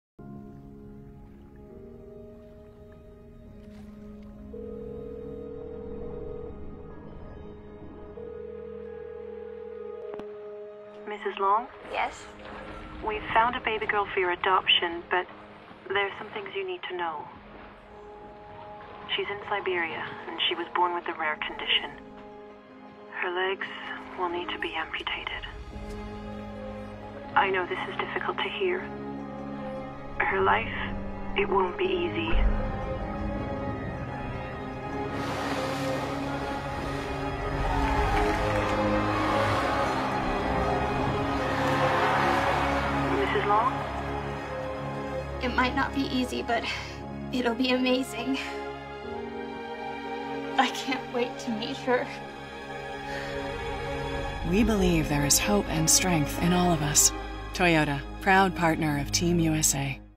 Jessica Long's Story
Toyota - Upstream

Agency: Saatchi & Saatchi LA
Director: Tarsem Singh
Starring: Jessica Long
Country: United States
Released: February 2021